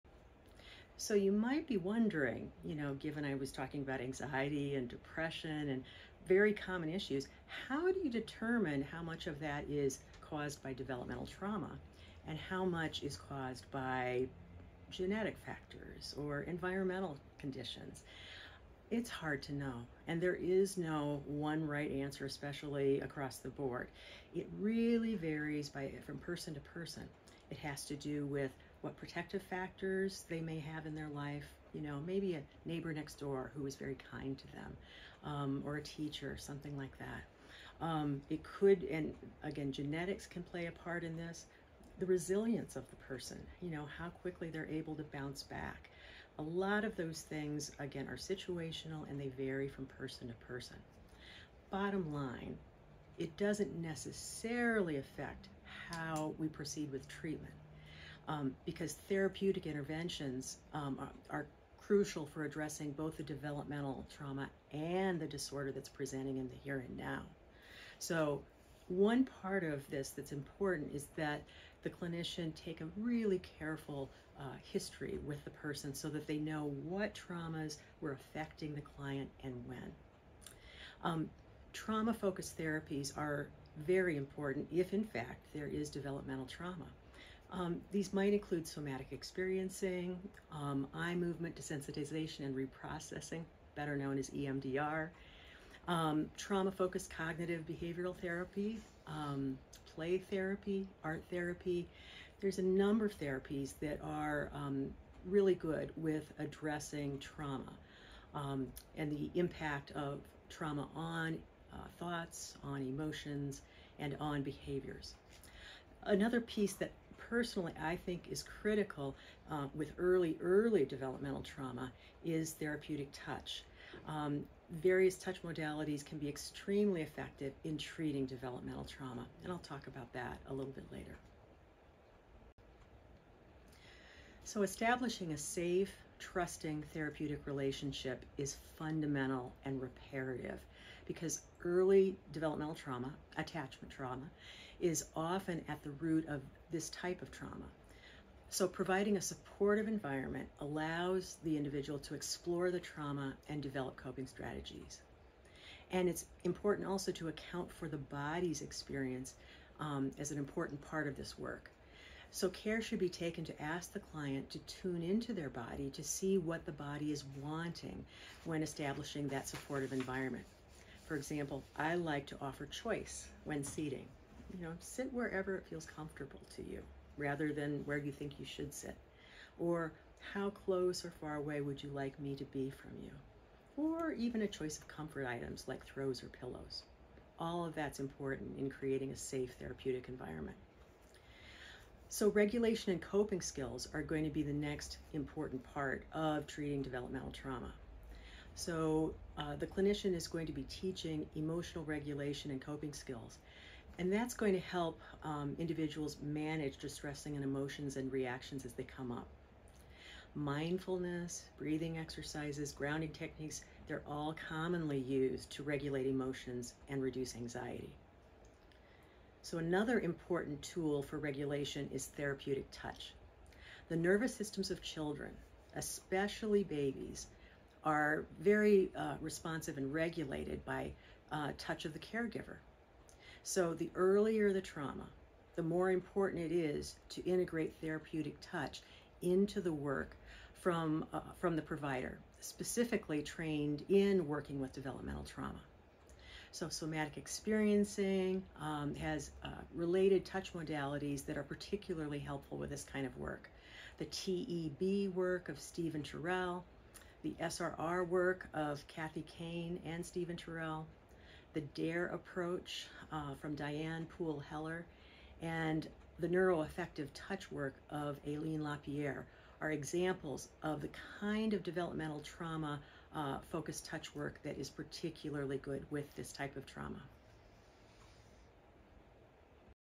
Treatment Approaches for Developmental Trauma
Rhonda discusses types of treatment used for Developmental Trauma